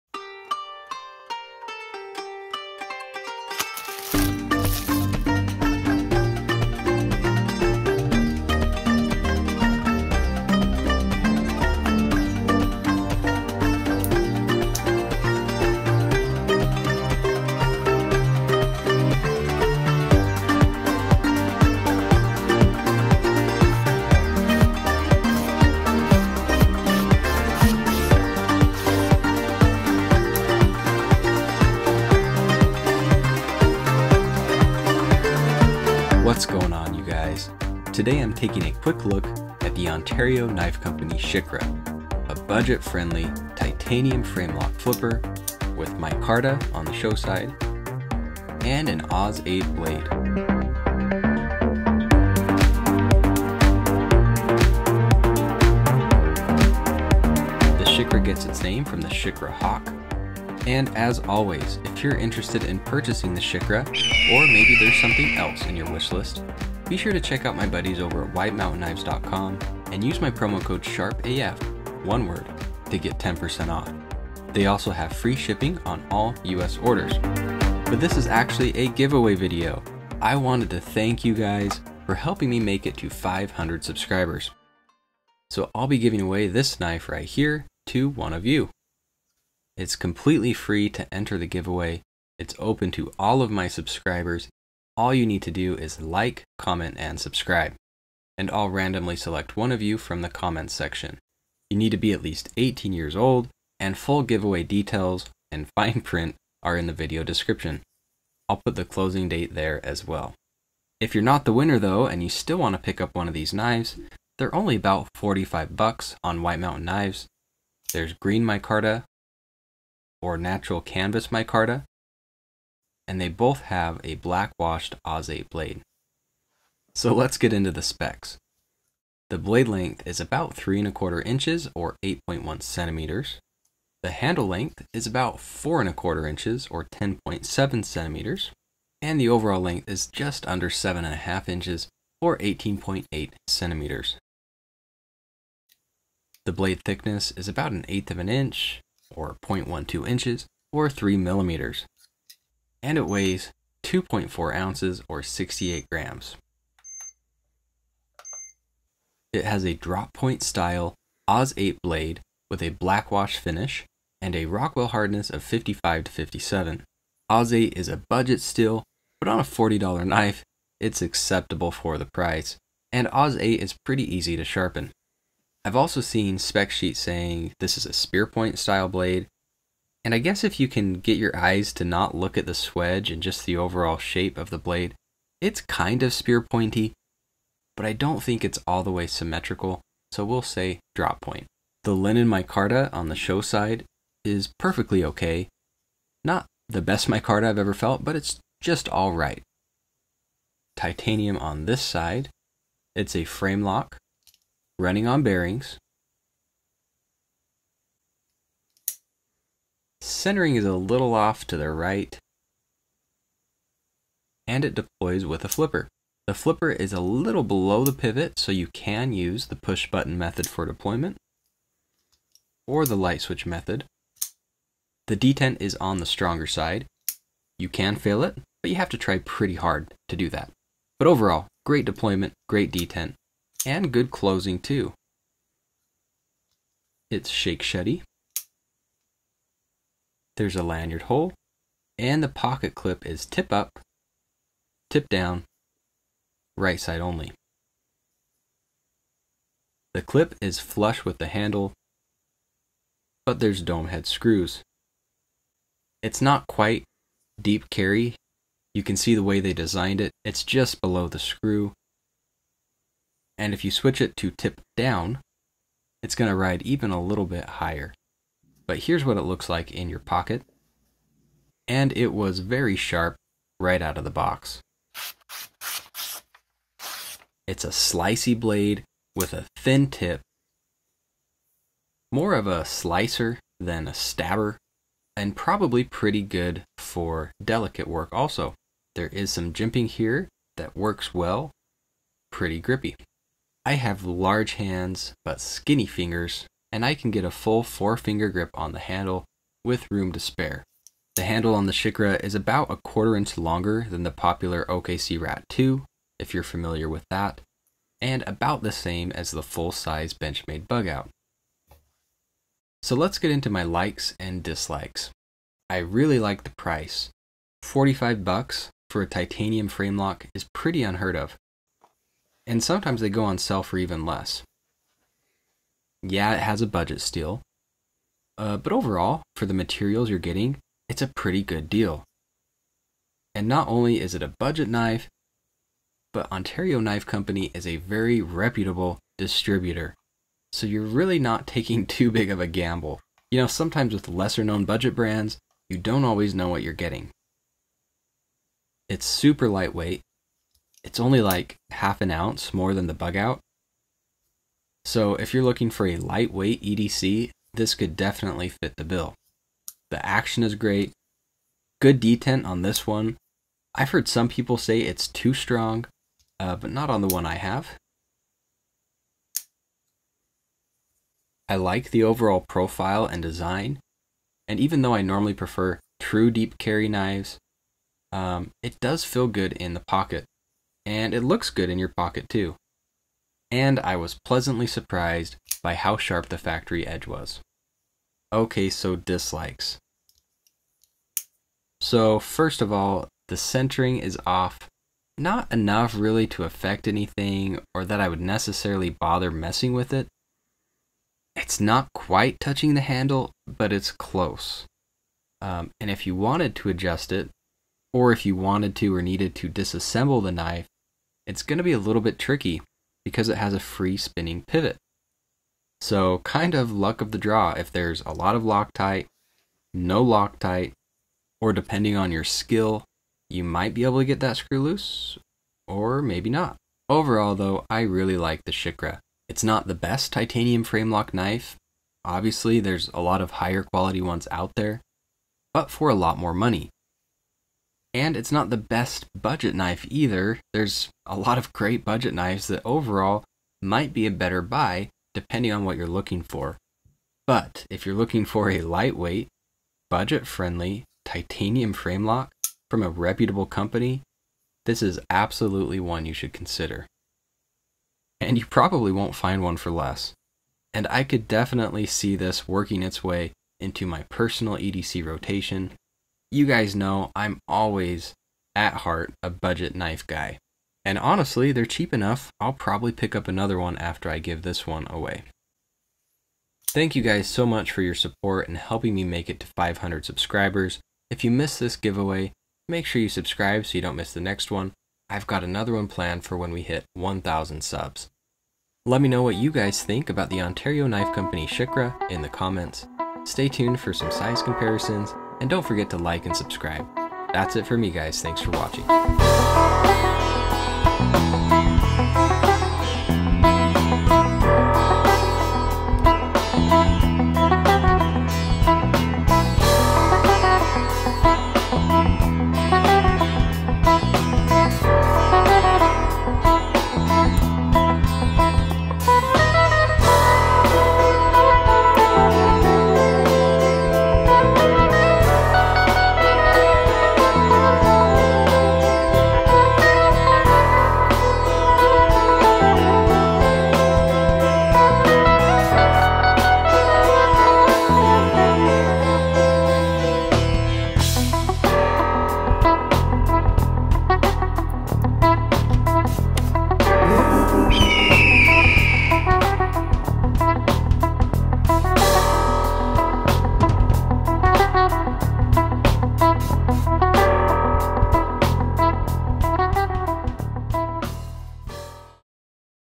Ontario Knife Company "Shikra" Overview and Giveaway!
Giveaway Details: This GAW is open from 3/8/22 - 5/31/22 I am giving away 1 knife to 1 subscriber. It is open to all of my subscribers 18 years or older. Leave a comment on the video to be entered and I will choose 1 person at random. You understand I am sending you a sharp pokey object that must be used with care and according to your local laws.  I am not responsible for any injuries etc and by participating in this giveaway you agree to not hold me liable for anything whatsoever. Use at your own risk! I reserve the right to run this GAW as I see fit but will do my best to be fair and transparent. I promise to SHIP the knife to the winner. But I cannot guarantee that it will arrive if it gets lost in transit or confiscated.. especially if you live in CANADA! I only have 1 knife to ship. But I will ship it to you for free! So pray to sweet baby jesus that it makes it to you. MOST IMPORTANT! ENJOY THE KNIFE!
-SHARP AF-

0:00 Unboxing / Music
0:36 Introduction / GAW
2:07 Specifications
5:12 Likes
6:28 Dislikes
8:13 Size Comparisons